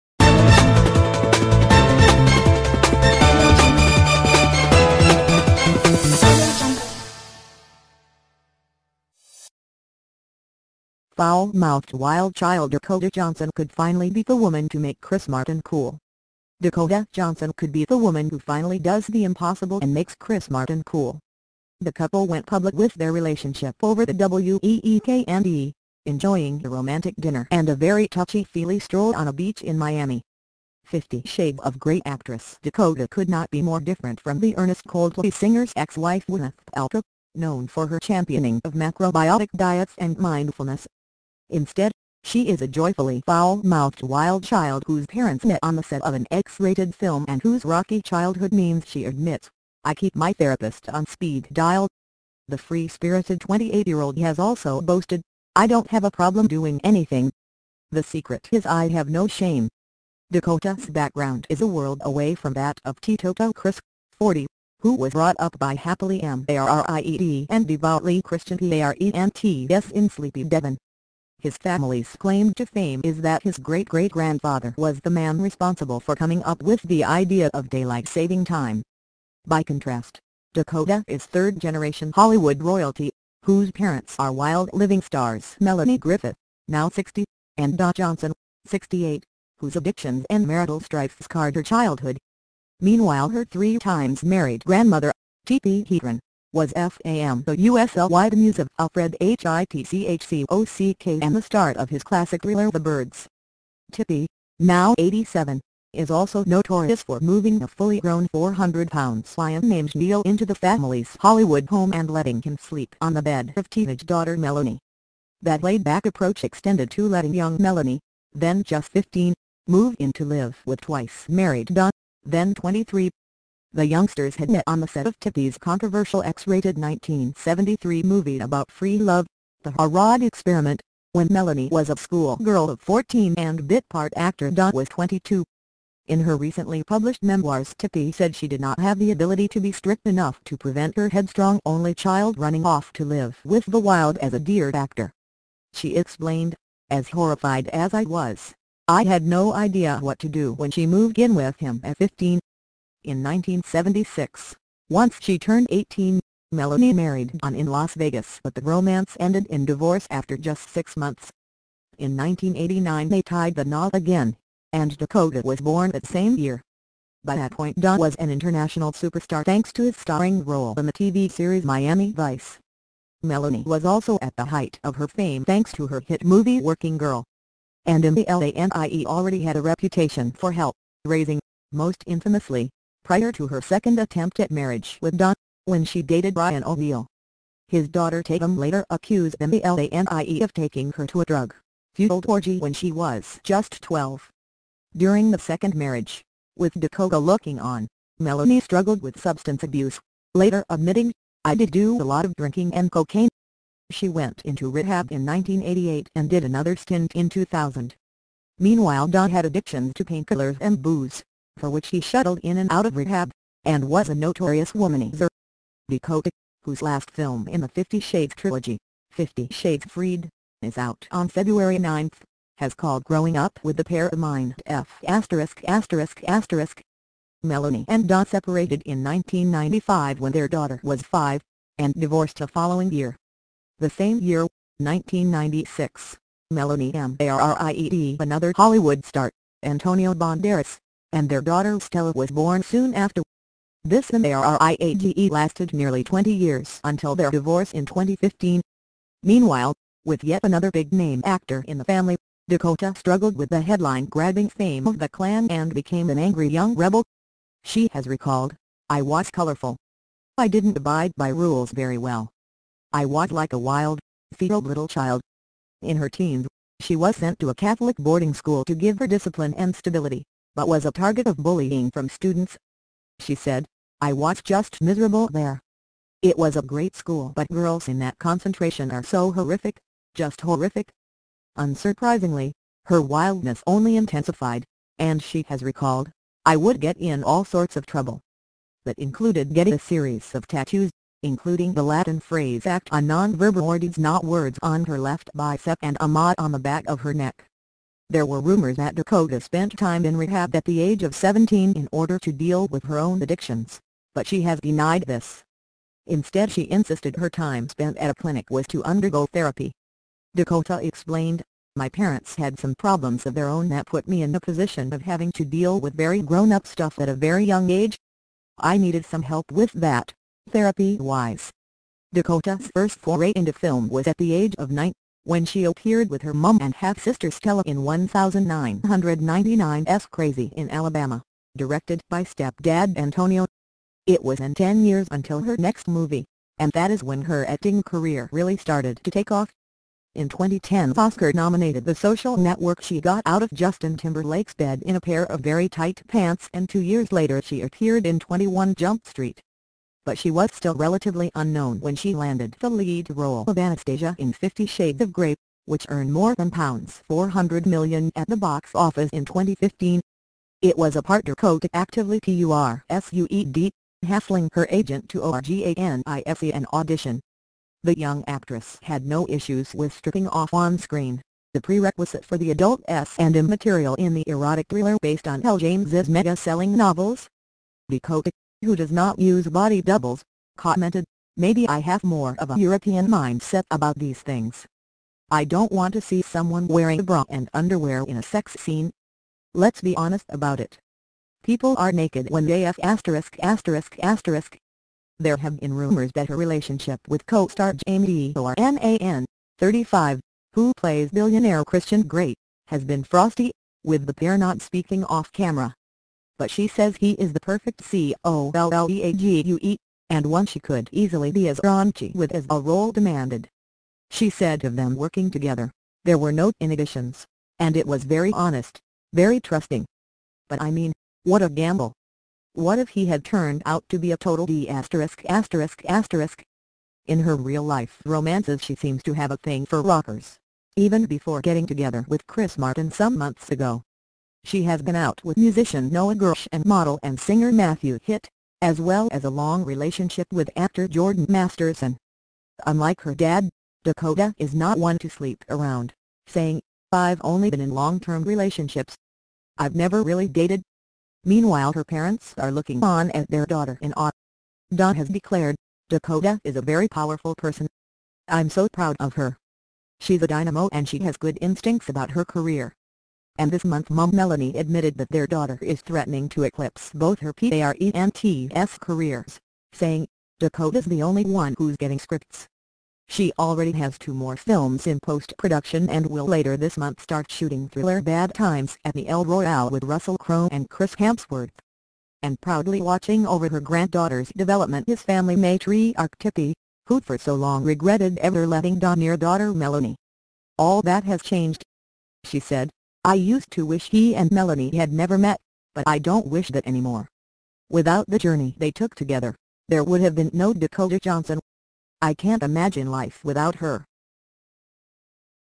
Dakota Johnson could be the woman who finally makes Chris Martin comfortable
The couple went public with their relationship over the ­weekend, enjoying a romantic dinner and a very touchy-feely stroll on a beach in Miami.